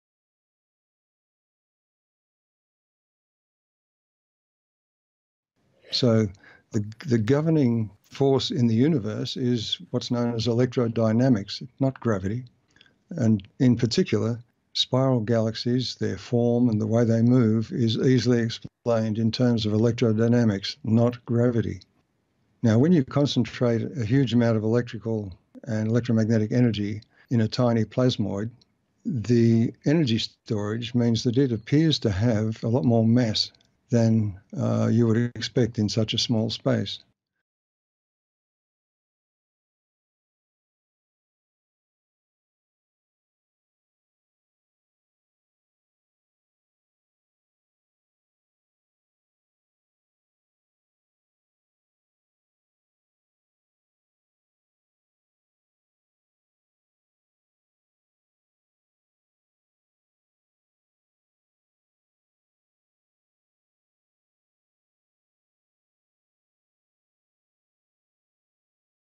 Wal Thornhill's comment on Plasmoids and Mass
Suggesting the hypothesis that M87 is NOT a Black Hole, but a powerful Plasmoid that generates Gravitational Induction
There is no accretion disk, the Plasmoid in the center of its galaxy IS the gravitational source.